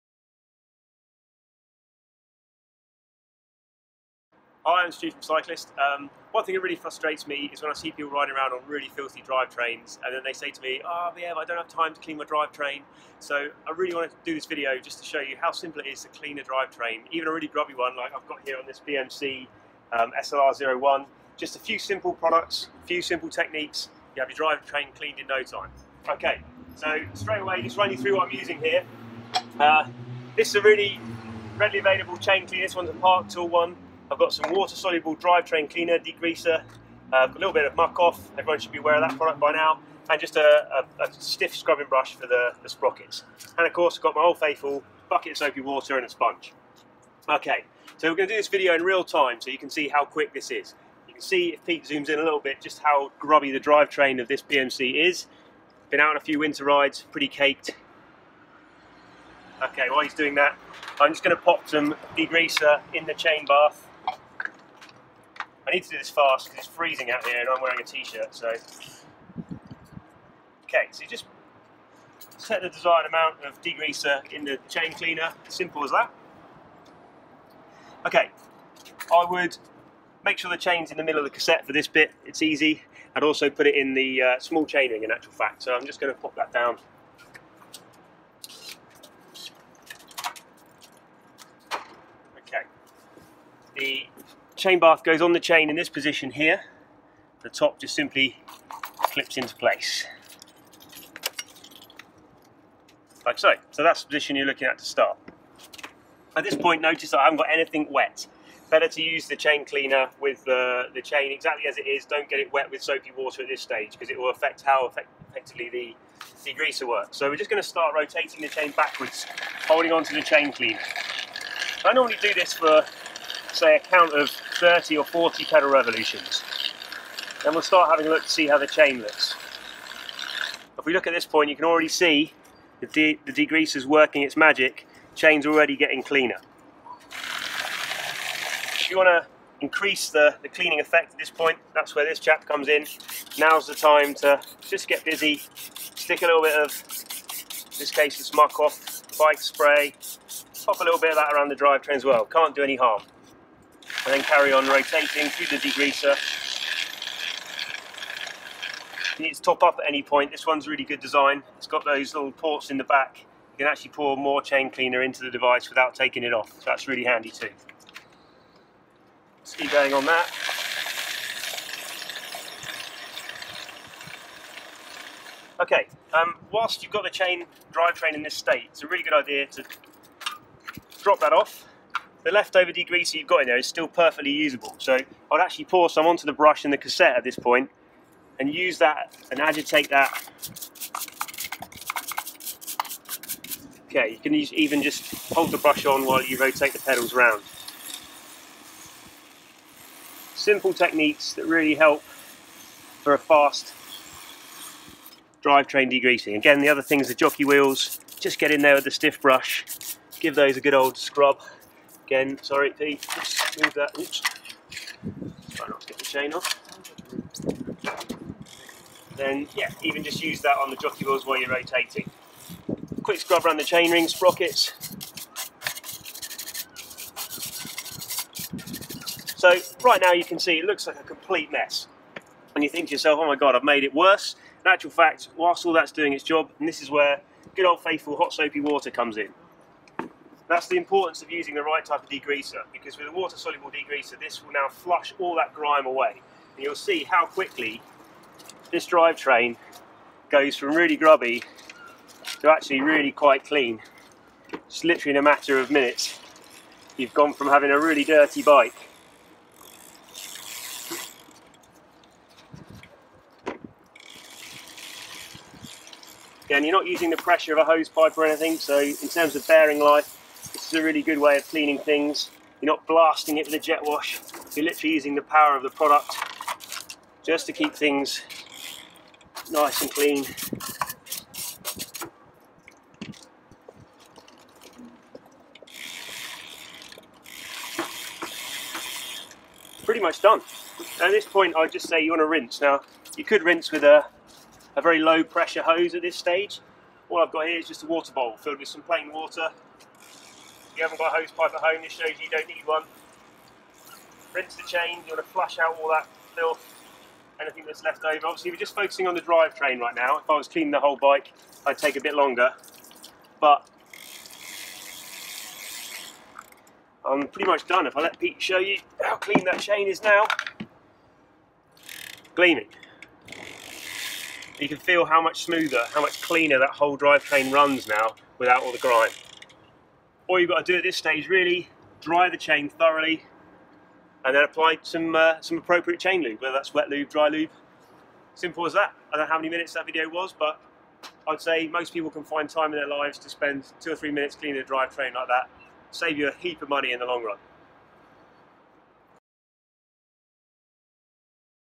How to clean your bike chain and drivetrain like a pro: Top tips for fast, efficient cleaning
There's no excuse for riding a bike with a dirty chain when it's this quick and simple to clean.

Read our full article on how to clean a chain

Submit your questions below👇

Also feel free to tell us: what new cycling trends/topics excite you? That way we can create more engaging content in the areas that interest you the most.